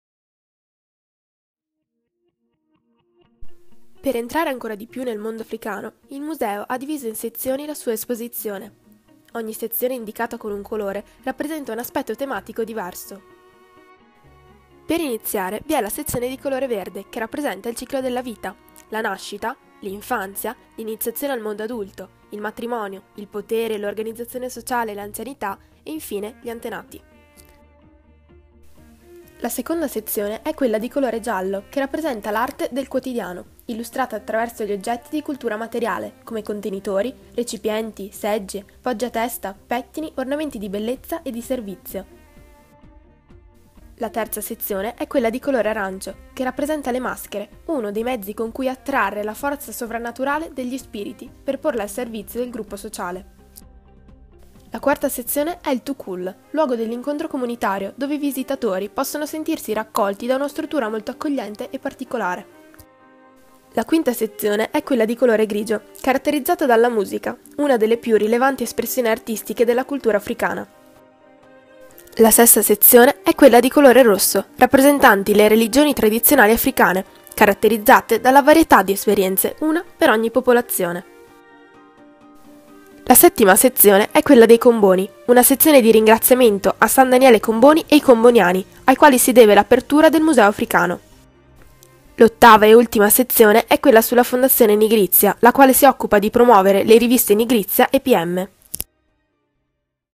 Esposizione permanente Museo Africano di Verona
Lavoro a cura della studentessa Helia Quattrocchi della Classe Terza B del Liceo Classico d’ordinamento nell’ambito dell’Alternanza Scuola Lavoro “Verona Minor Hierusalem”